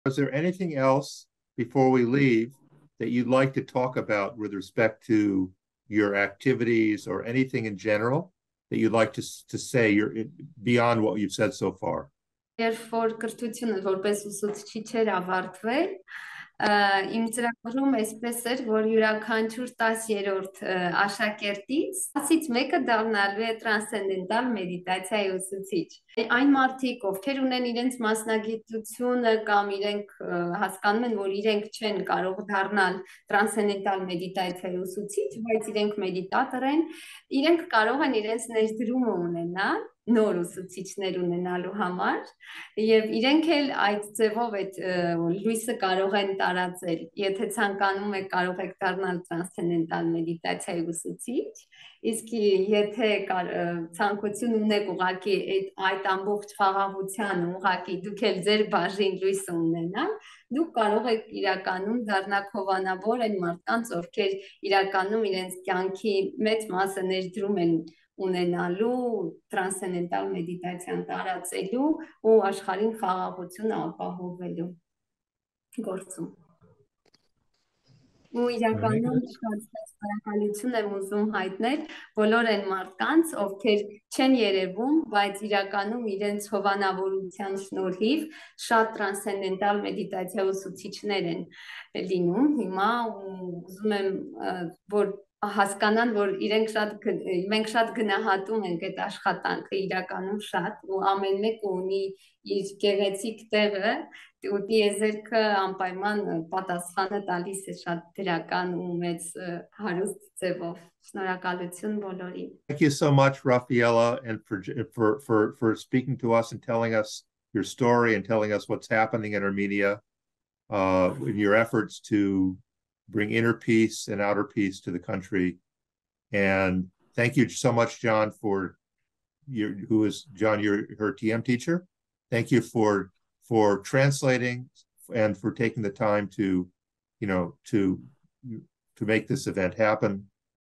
Invitation to help bring inner peace as a teacher of Transcendental Meditation
New TM Teacher Rafayela Bagratuni invites everyone to learn evidence-based Transcendental Meditation and become a TM teacher to bring inner peace to your nation.